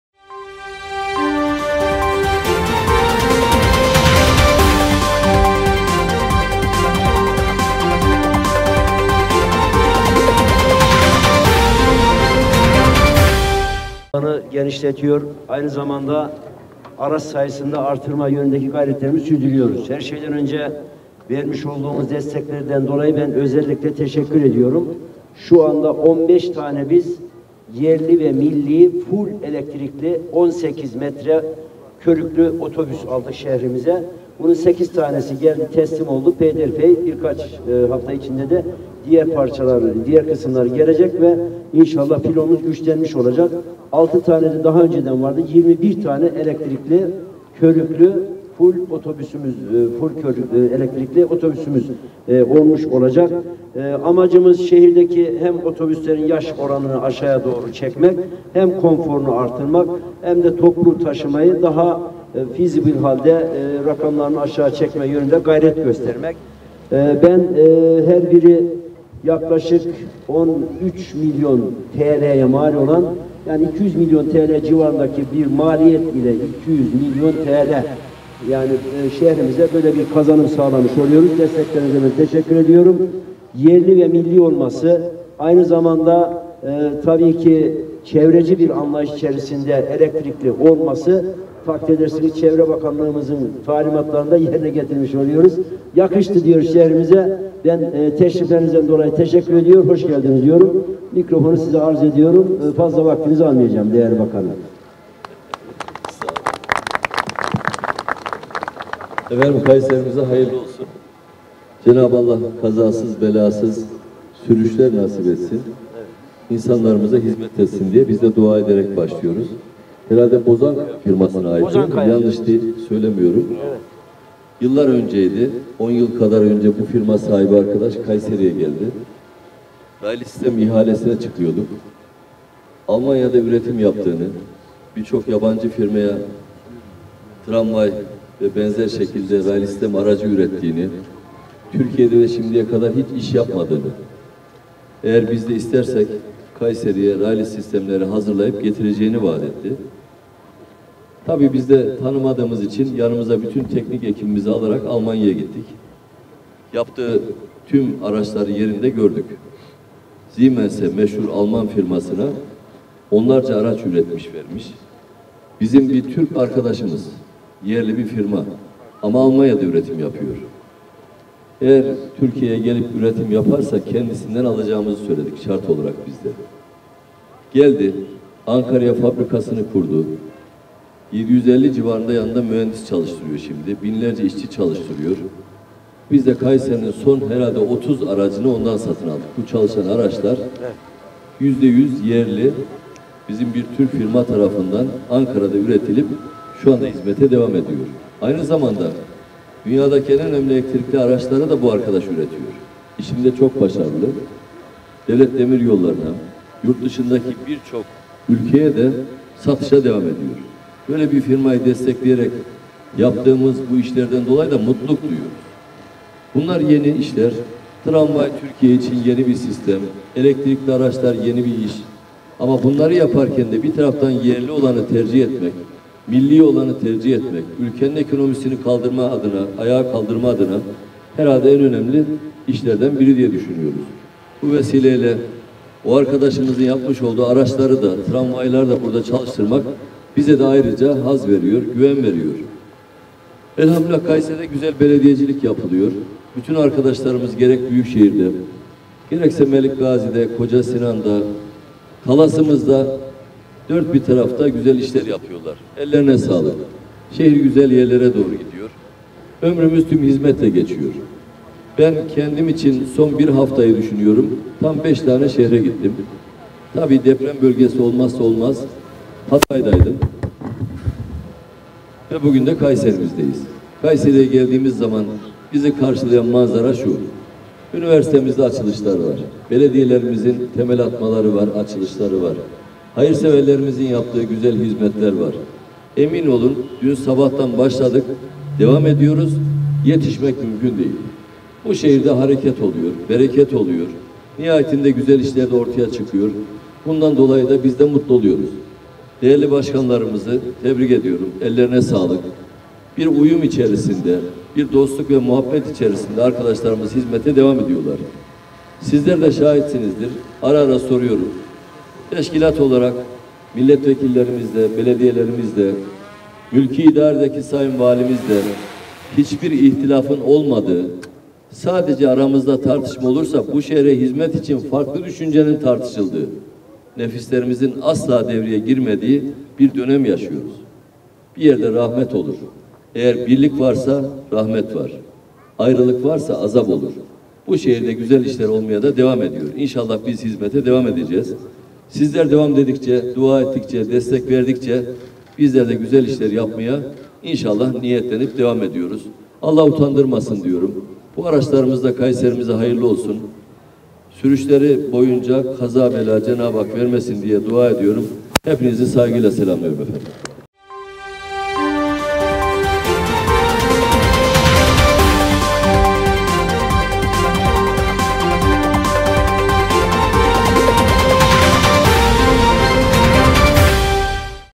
Kayseri BB Elektrikli Otobüs Teslim Törenine Çevre ve Şehircilik Bakanı Mehmet Özhaseki'de Katıldı
Kayseri Büyükşehir Belediyesi Elektrikli Otobüs Teslim Törenine Çevre ve Şehircilik Bakanı Mehmet Özhaseki'de Katıldı.